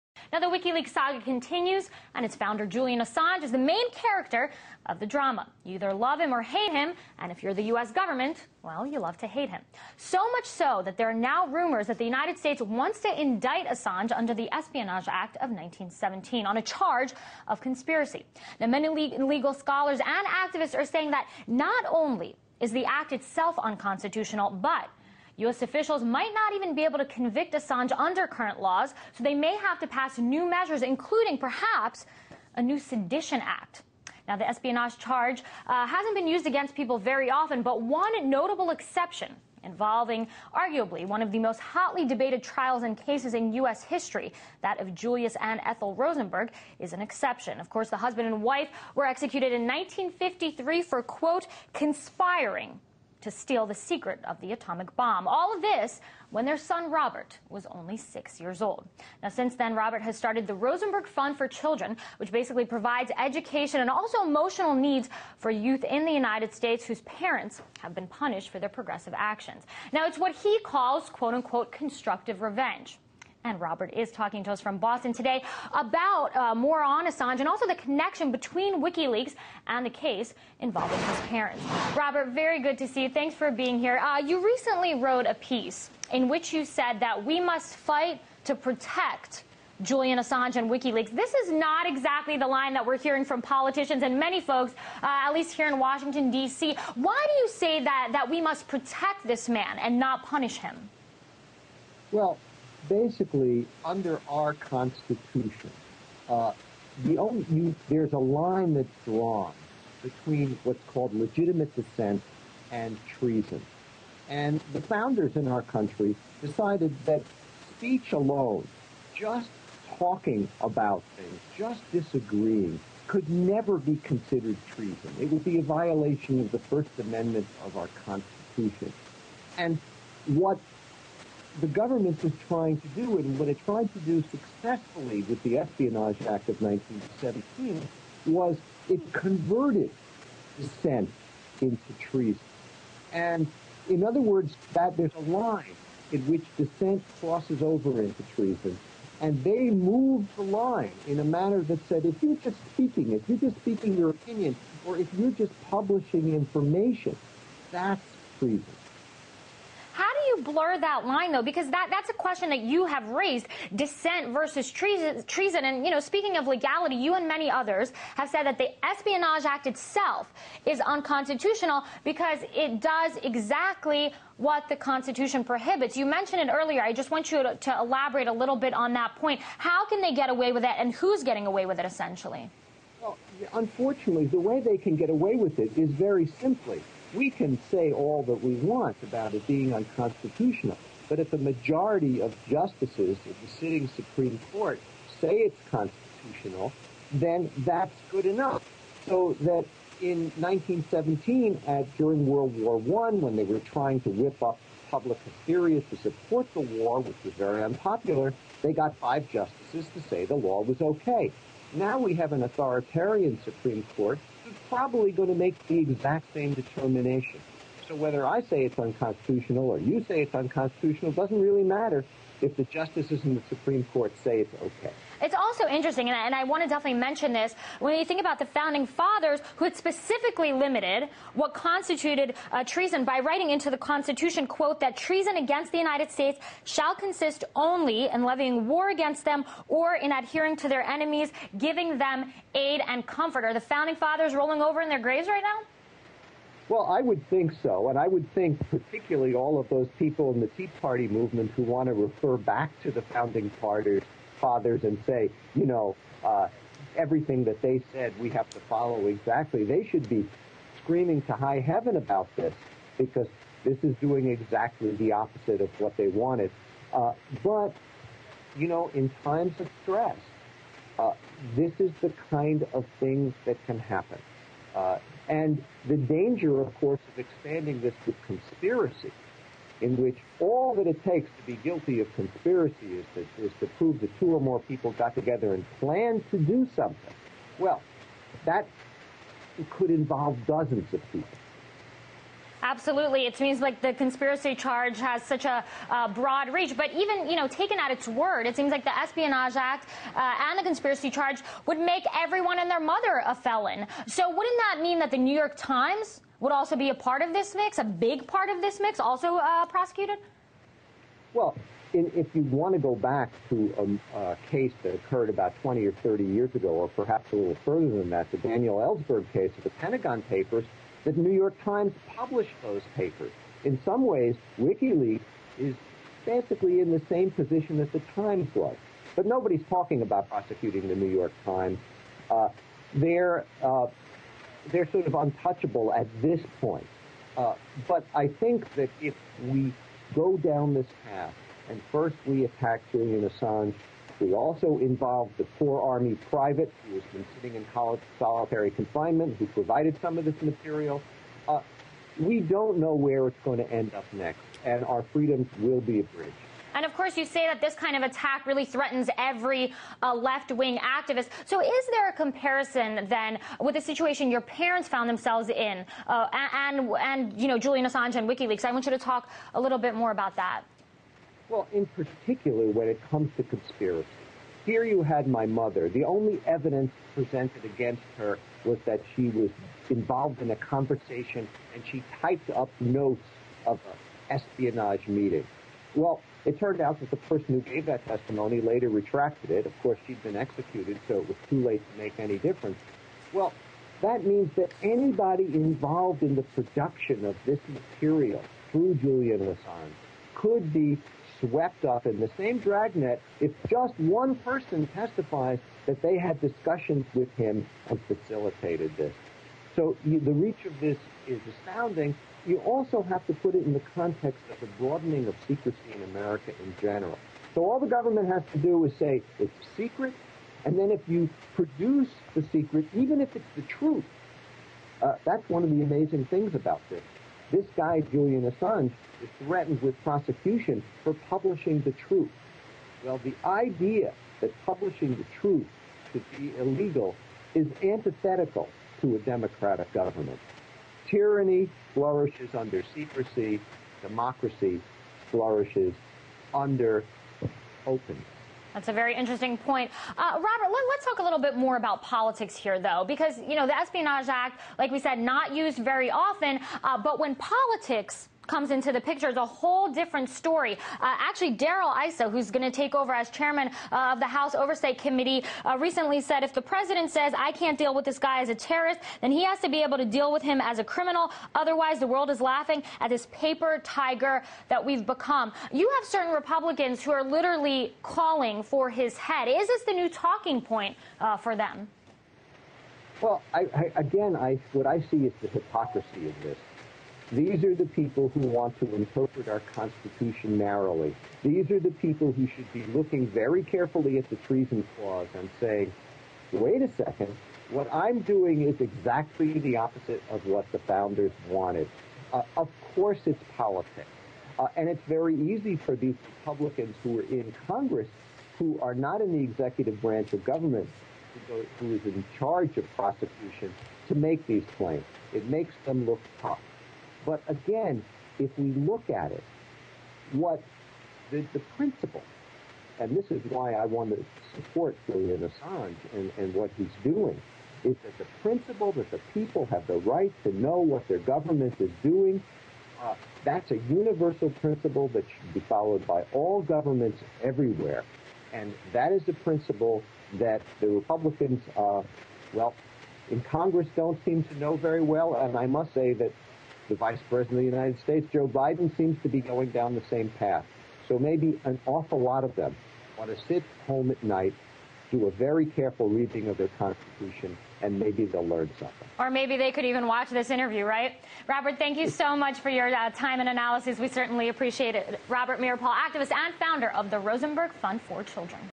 The fight to protect Julian Assange
01/04/10) His parents executed under the Espionage Act of 1917, Robert Meeropol speaks out against the indictment of Julian Assange under this unconstitutional law.